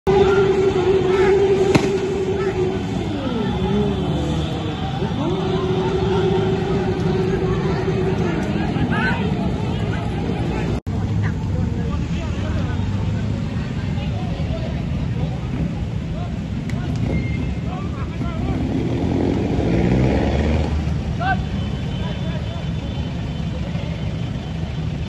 ตำรวจเคลียร์พื้นที่หน้าศาลฎีกา-หลังแกนนำราษฎรยุติชุมนุม | 13 ก.พ. 64
13 ก.พ. 64 ตำรวจเคลียร์พื้นที่บริเวณถนนราชดำเนินใน ฝั่งศาลฎีกา หลังแกนนำราษฎร 63 ยุติชุมนุม อย่างไรก็ตามมีมวลชนบางส่วนไม่ยอมกลับ มีการตอบโต้กับเจ้าหน้าที่ตำรวจ โดยตำรวจจับกุมผู้ชุมนุมอย่างน้อย 11 ราย